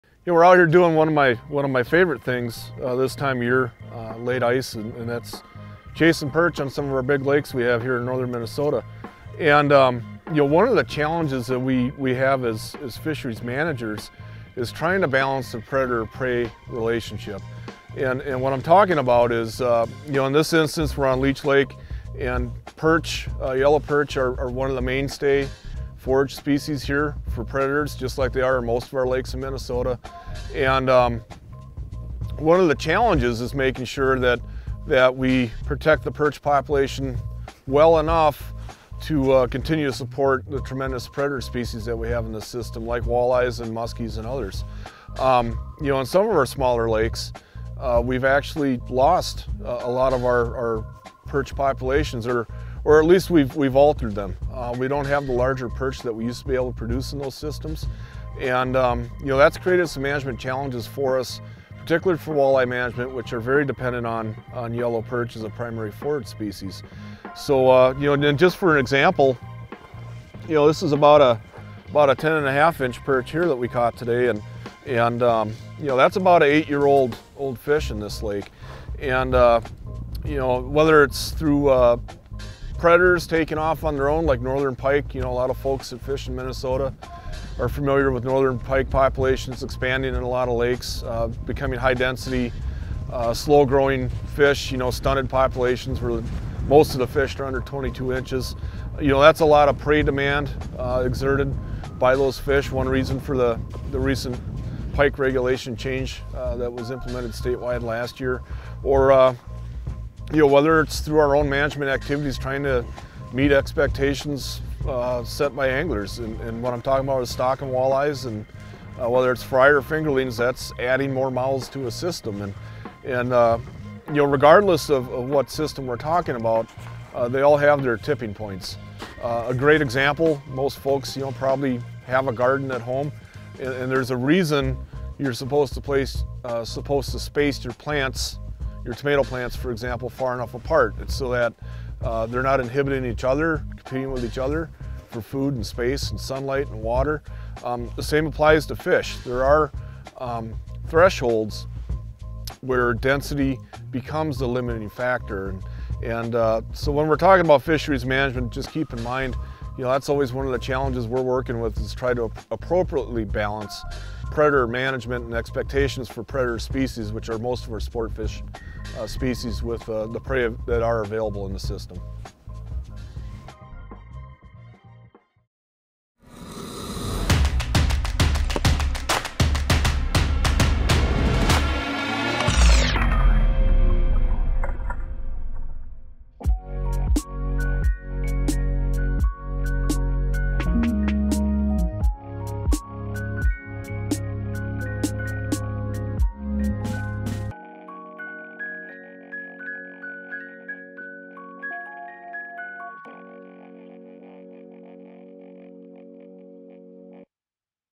Why the Predator/Prey Balance Matters (Science)
Minnesota DNR Area Fisheries Supervisor Doug Schultz explains the importance of monitoring the predator prey balance in fisheries like Leech Lake.

Don't forget to grab your ice fishin' gear at your local Fleet Farm store or online: